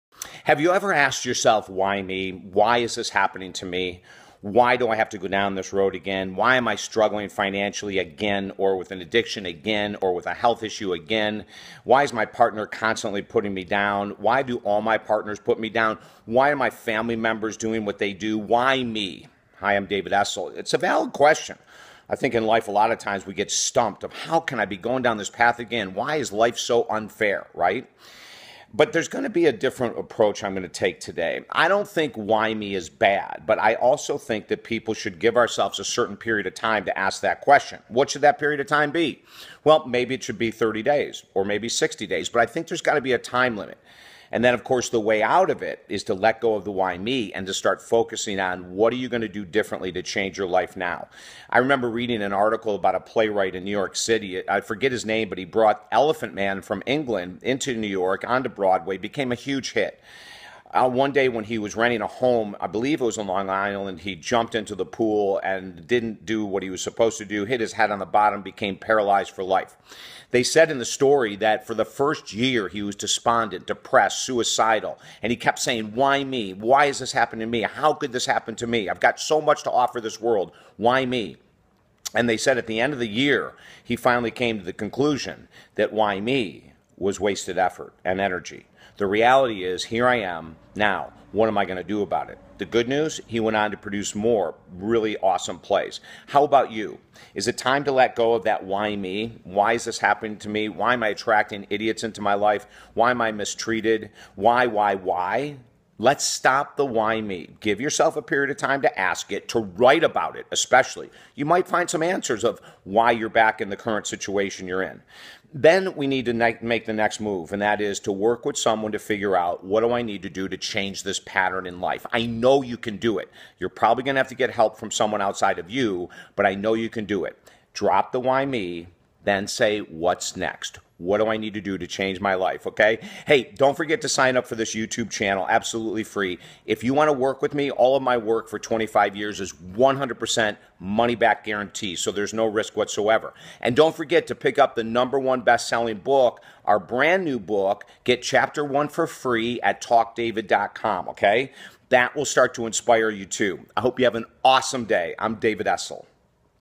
VIDEO 739. WHY, is this happening to me? David Essel
If you see things happening in life that are not positive, you may ask… Why me? Watch this video, let's heal together. Love David